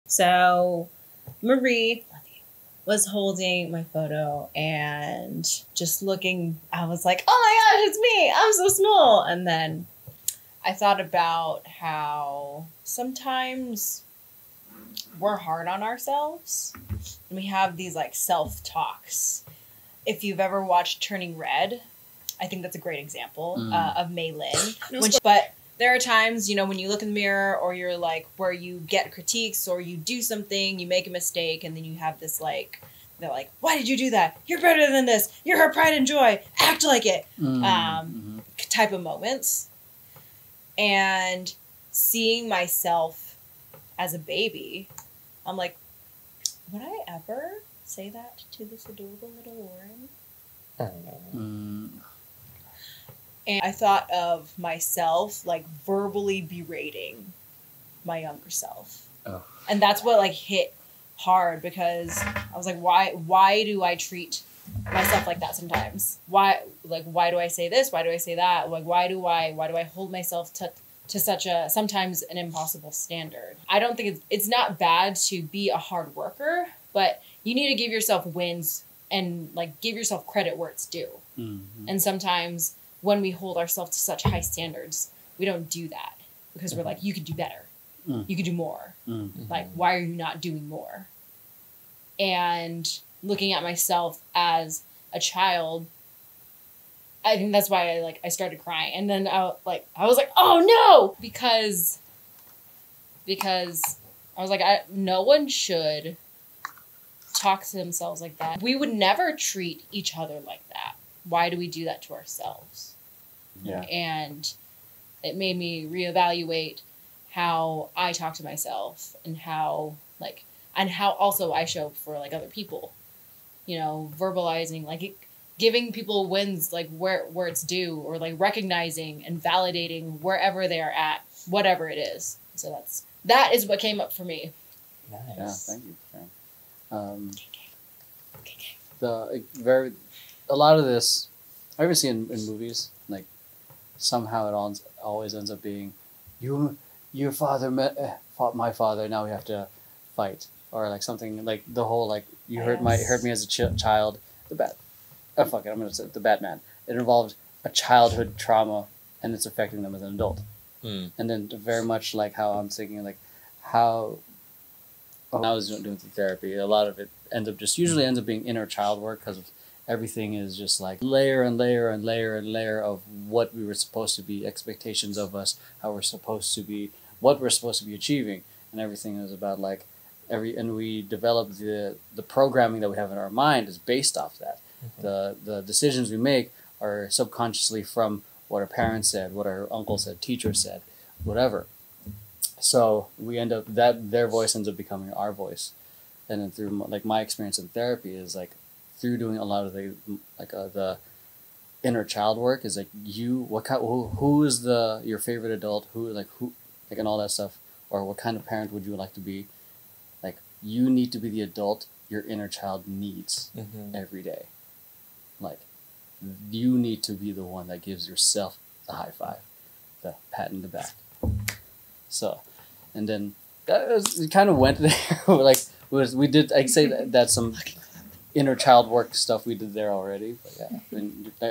Self-Talk Sabotage: Why We Need to Stop Being Our Own Worst Critics
In this episode, Lauren reflects on her self-talk and how it affects her mental health. She shares a vulnerable moment of seeing a photo of herself as a baby and realizing the harsh way she sometimes talks to herself. Through her journey in therapy and inner child work, Lauren has learned the importance of giving herself wins and recognizing her achievements. Join us as we explore the power of self-talk and inner child work.

In today's episode we discuss about:

+Reflection on self-talk and its impact on mental health
+Vulnerable moment of seeing a photo of herself as a baby and realizing harsh self-talk
+Importance of giving yourself wins and recognizing achievements
+Exploration of inner child work and how it affects our programming and decision-making
+Learning to be the adult our inner child needs every day